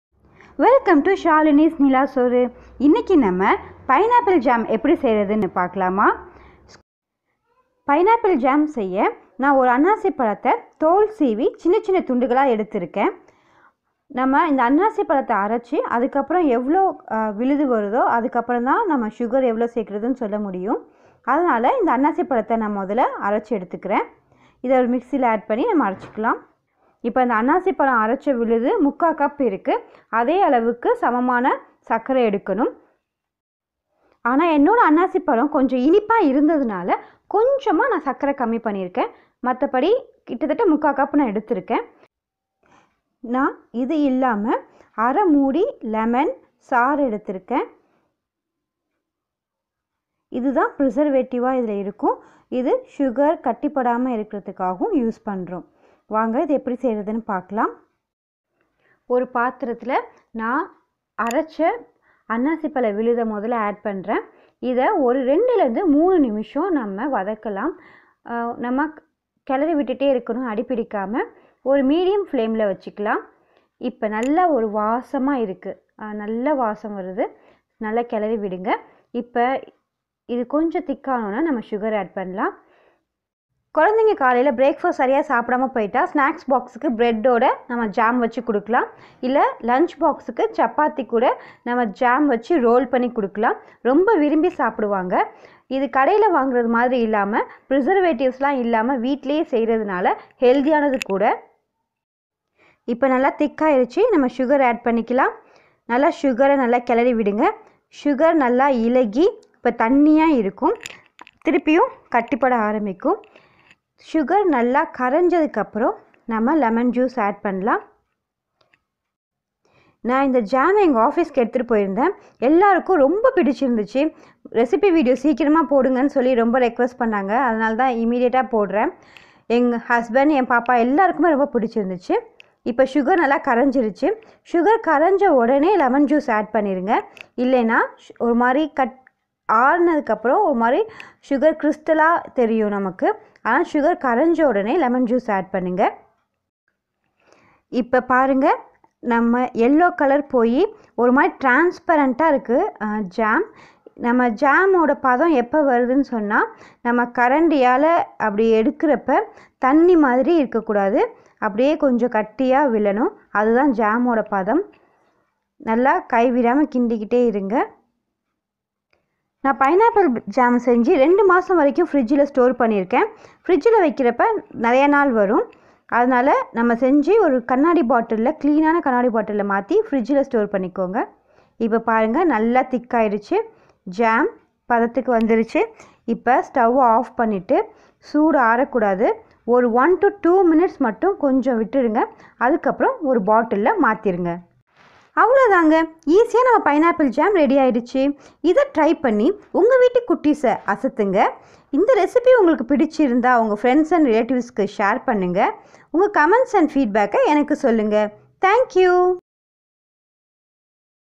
Recipe 17 - Pineapple Jam / அன்னாசி பழ ஜாம்
Friends,

Let us prepare a healthy jam with no preservatives..

Note:

1. The Entire process must be done in medium flame.
2. It should be transfered into clean and dry glass bottle when it is hot.
3. Close the lid once it comes to the room temperature.
4. I have stored it in fridge for about 2 months.

Ingredients:

Pineapple - 1 full
Sugar - according the measurement of pineapple pulp
Lemon - 1/2

Steps:
1. Peel the outer part of the pineapple and chop into pieces.
2. Grind the pineapple pieces into a pulp and measure it.
3. Take the same quantity of sugar.
4. Take a pan and add the pineapple paste and cook for 2 to 3 minutes.
5. Once it is slightly thicken, add the sugar.
6. Let the sugar dissolve.
7. Then add lemon juice.
8. Stir continously so that it should not burn.
9. After 7 to 10 mins it will be thick like jam.
10. Switch off the stove.